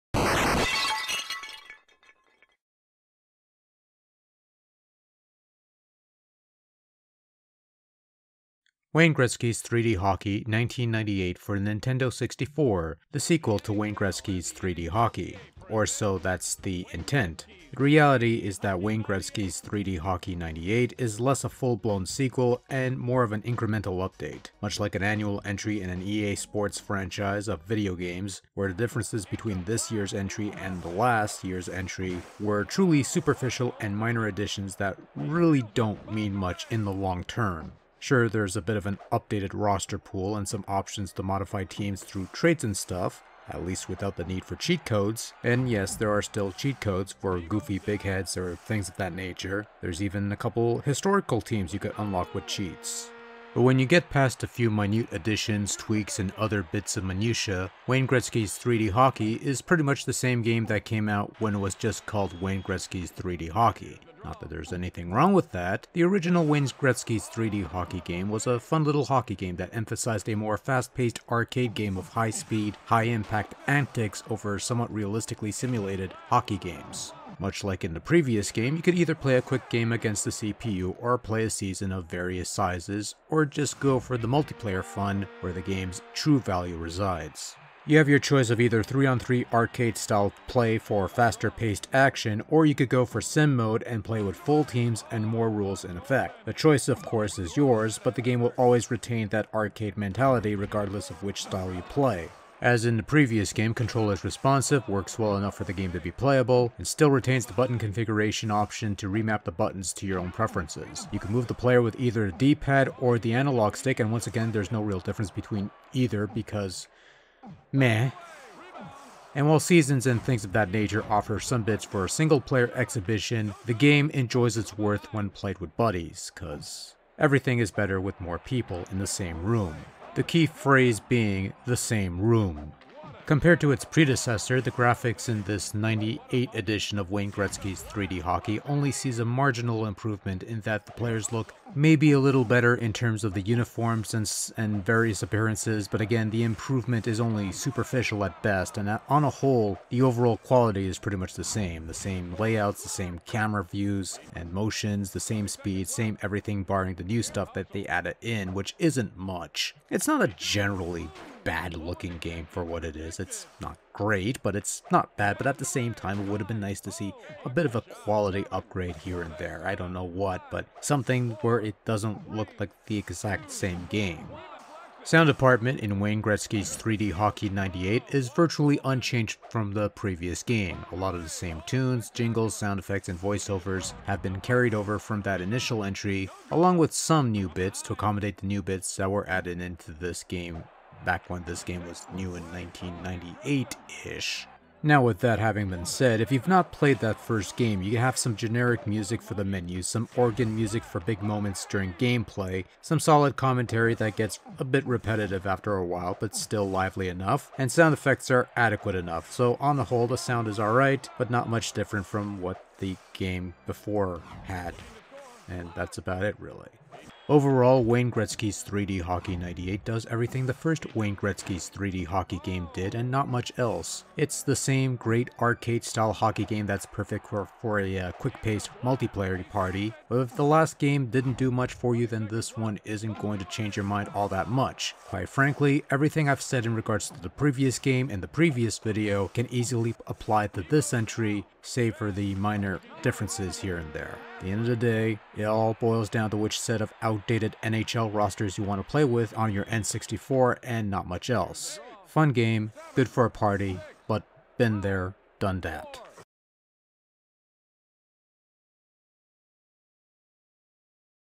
Review 619 - Wayne Gretzky's 3D Hockey 98 (N64)
A video review of Wayne Gretzky's 3D Hockey 98 for Nintendo 64.

The second of two reviews to be posted today.